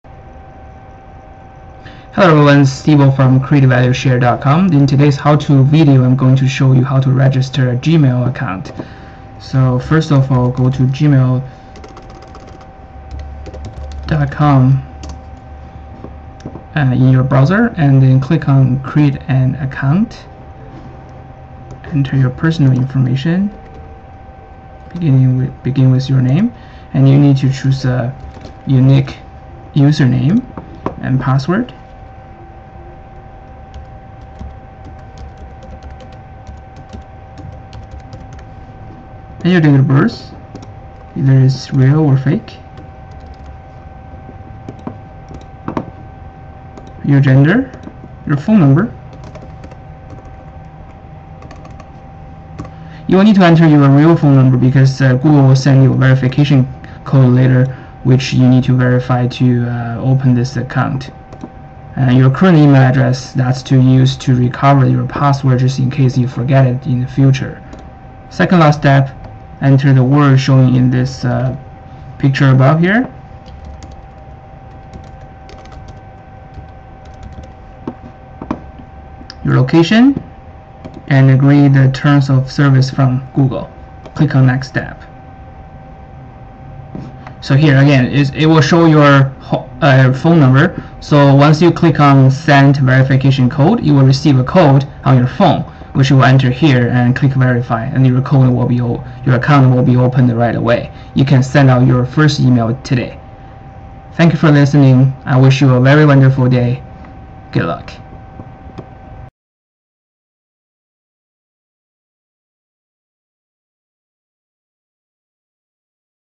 How to register a gmail account
Here in this video I show you how to register a gmail account.